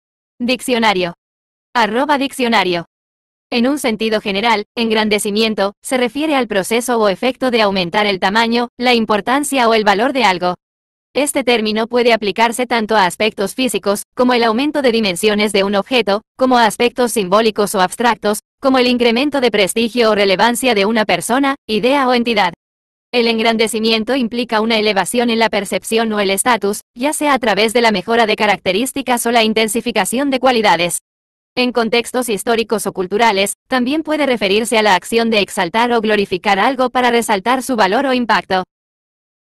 Engrandecimiento Significado En Español
En un sentido general, "engrandecimiento" se refiere al proceso o efecto de aumentar el tamaño, la importancia o el valor de algo. Este término puede aplicarse tanto a aspectos físicos, como el aumento de dimensiones de un objeto, como a aspectos simbólicos o abstractos, como el incremento de prestigio o relevancia de una persona, idea o entidad. El engrandecimiento implica una elevación en la percepción o el estatus, ya sea a través de la mejora de características o la intensificación de cualidades. En contextos históricos o culturales, también puede referirse a la acción de exaltar o glorificar algo para resaltar su valor o impacto.